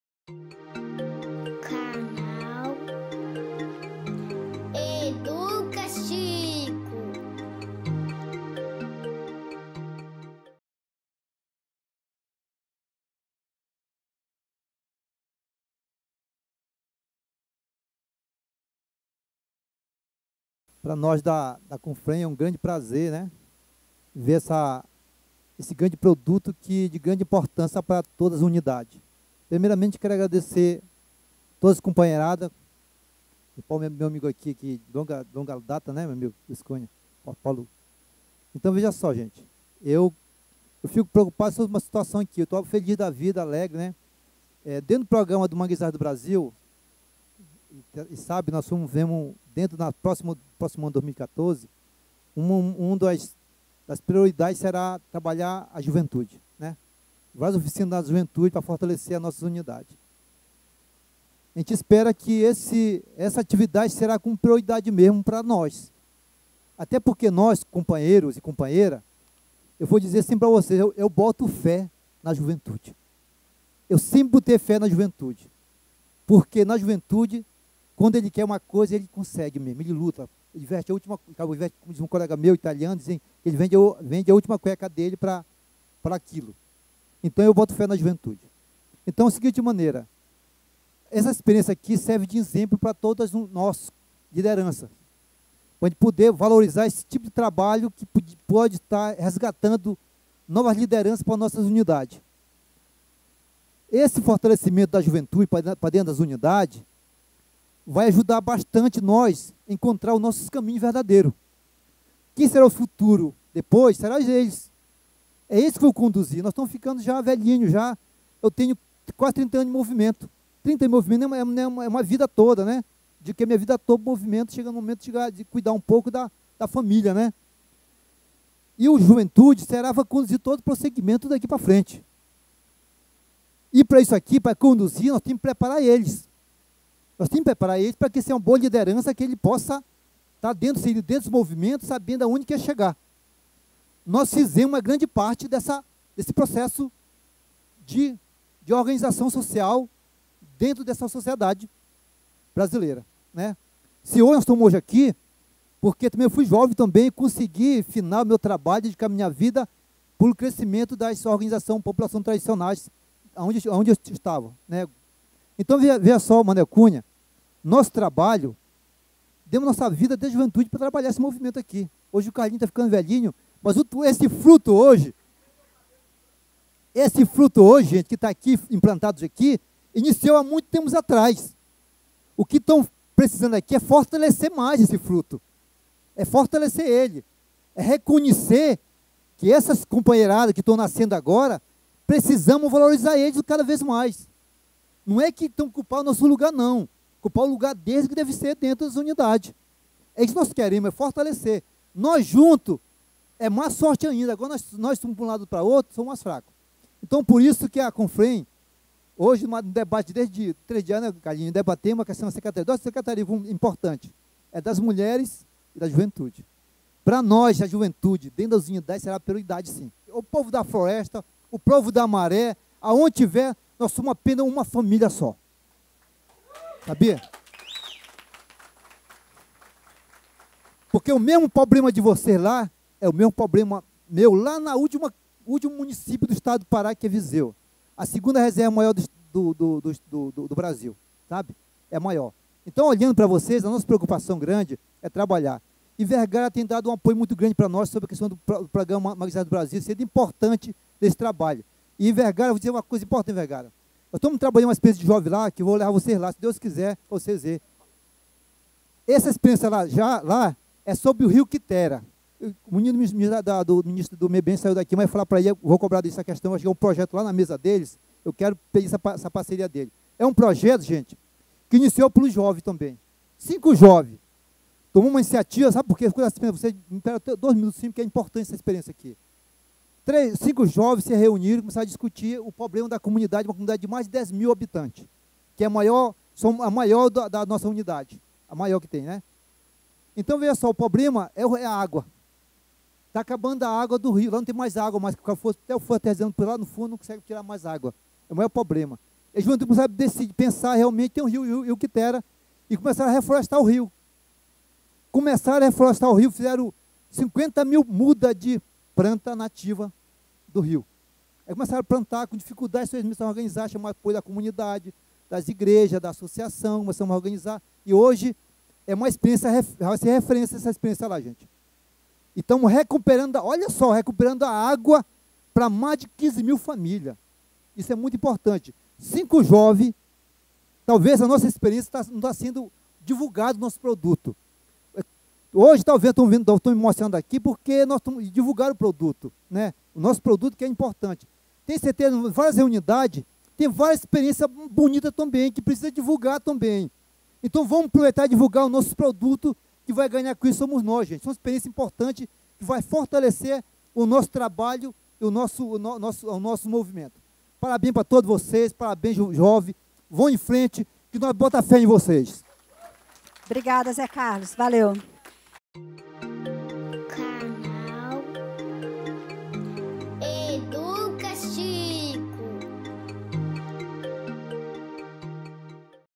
José Carlos (CONFREM) - Trecho de debate sobre o Projeto Jovens Protagonistas - auditório do ICMBio
Trecho do debate realizado no auditório do ICMBio, no dia 17 de dezembro de 2013, durante lançamento de vídeo "Jovens como Protagonistas do Fortalecimento Comunitário"

Projeto "Jovens como Protagonistas do Fortalecimento Comunitário"